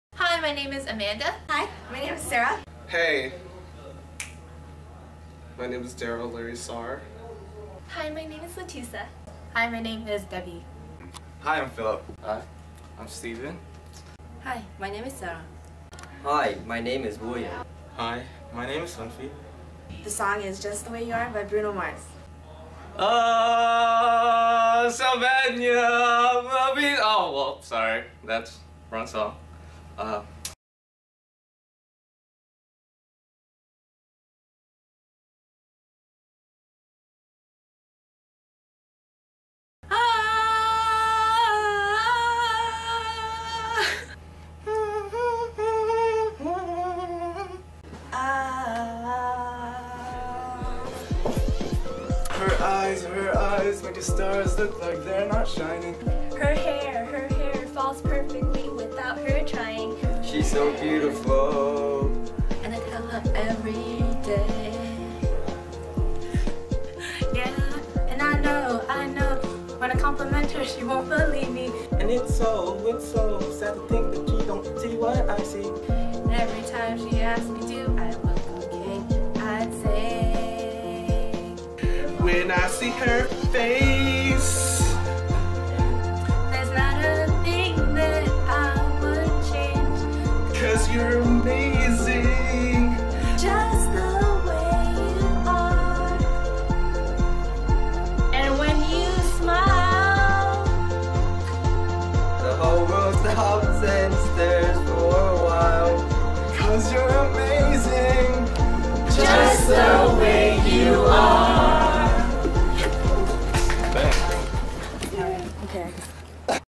CAO Culture Night 2013 Board
Cambodian Awareness Organization (CAO) at UC Irvine board video shown during the 4th Annual Charity Culture Night "Homecoming: Uniting the Past and Present".